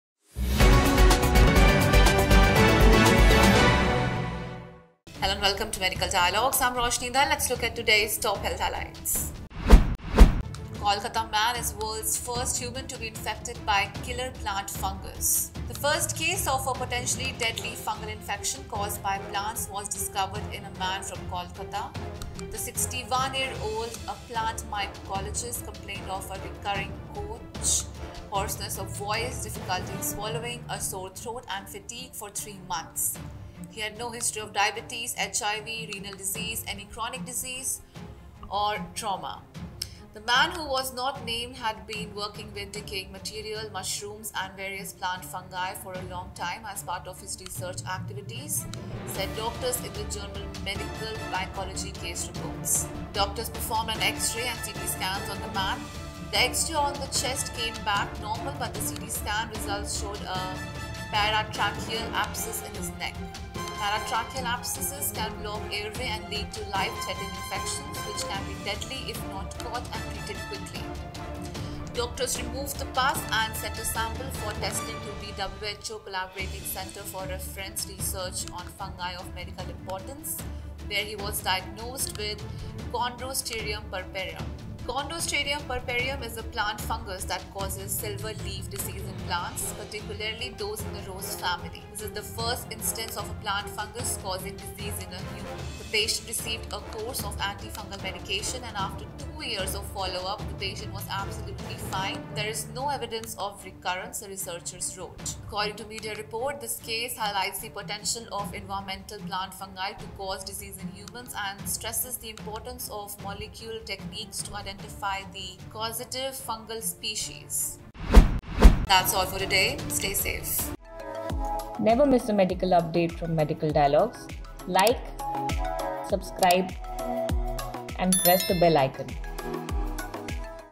World's first human to be infected by killer plant fungus in Kolkata
The first case of a potentially deadly fungal infection caused by plants was discovered in a man from Kolkata. The 61-year-old, a plant mycologist, complained of a recurring couch, hoarseness of voice, difficulty swallowing, a sore throat, and fatigue for three months.

Doctors removed the pus and sent a test sample to the "WHO Collaborating Centre for Reference & Research on Fungi of Medical Importance", where he was diagnosed with Chondrostereum purpureum.